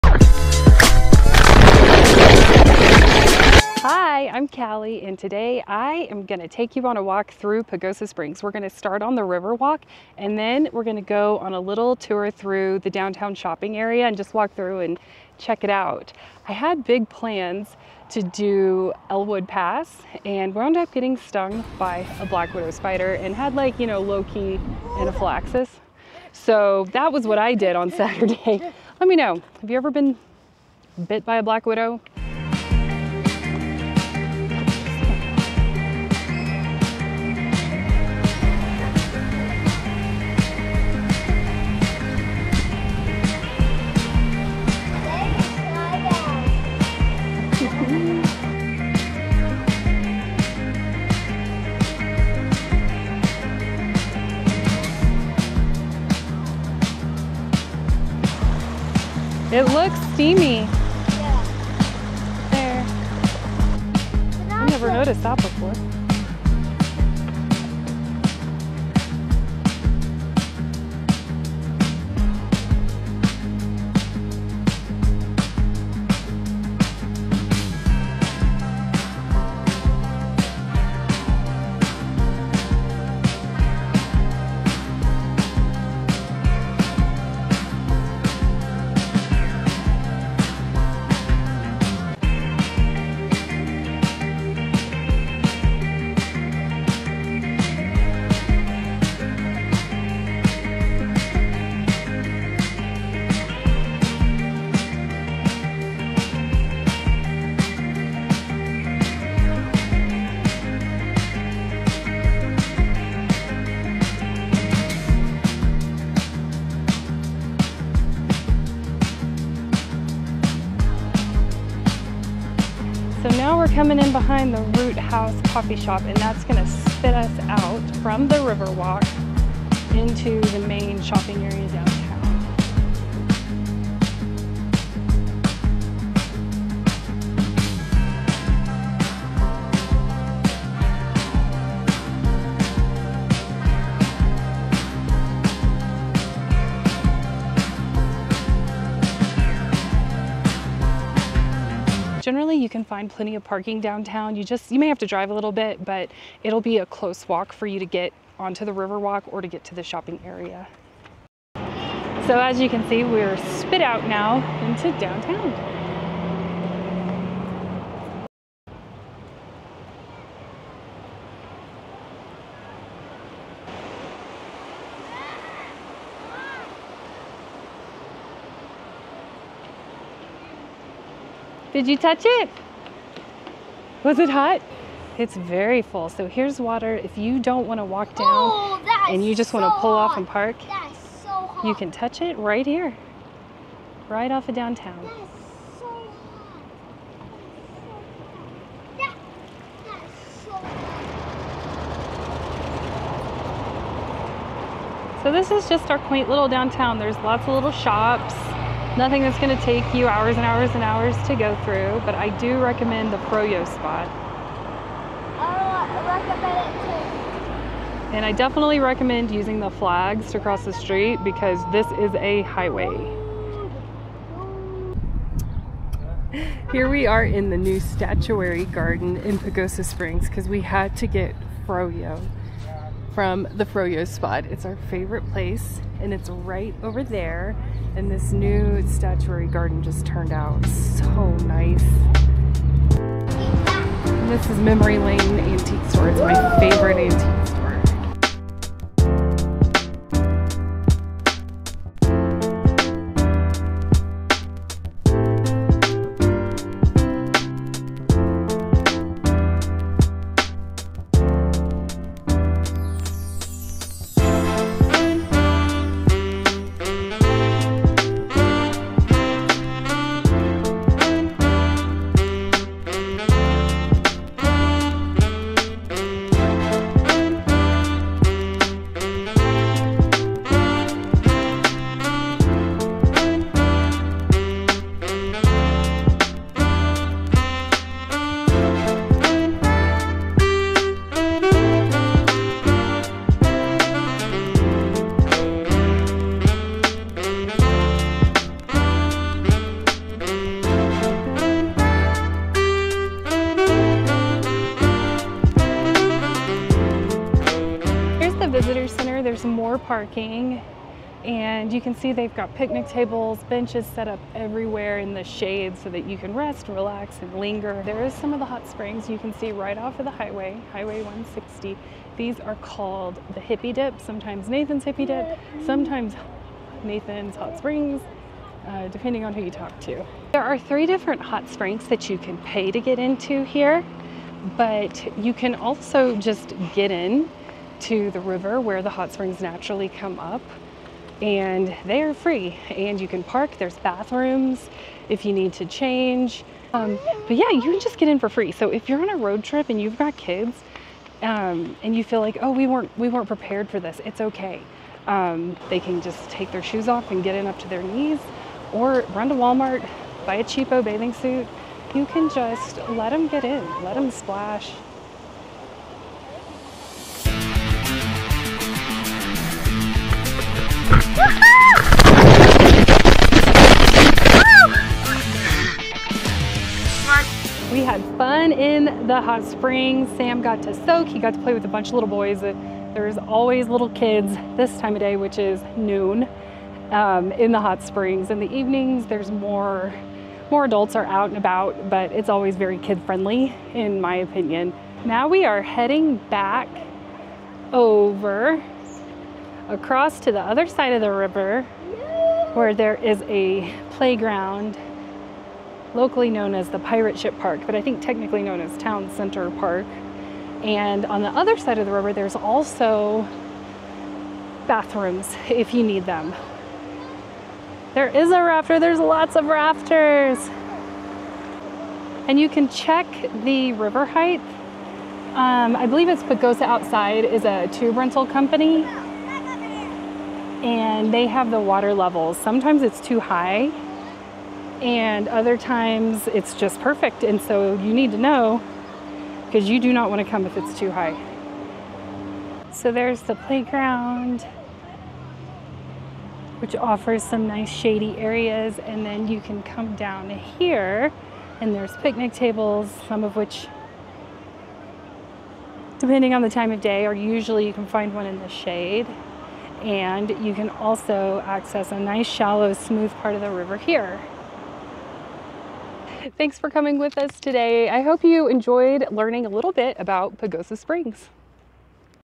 Tour Pagosa Springs: Riverwalk, Hot Springs, and More!
Come play with us in Pagosa Springs, Colorado! Learn how to have fun for free in Pagosa! We explore the river walk, soak in the hot springs, and tour downtown.